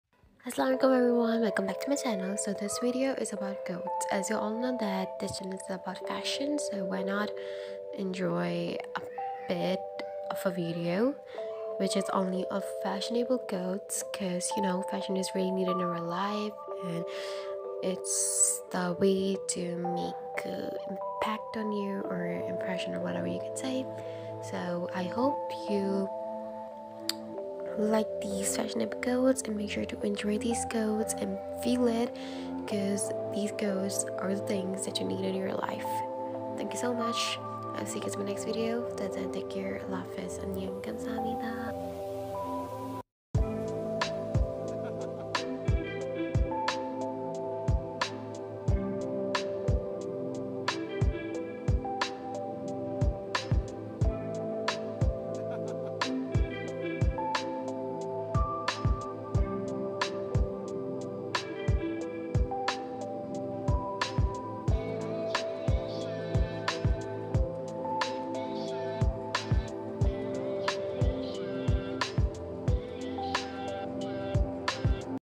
Fashion quotes for girls
dear friends hope you all are well.

The content of this video is kinda different. it's about fashionable quotes. We need fashion in our life to make this world more beautiful by your elegant and colorful personality. That's why fashion is what we need to make our life more beautiful. You can say that this video is like a introductory video of my channel~

Hoping that it would be great for you to reas these beautiful fashionable quotes 💜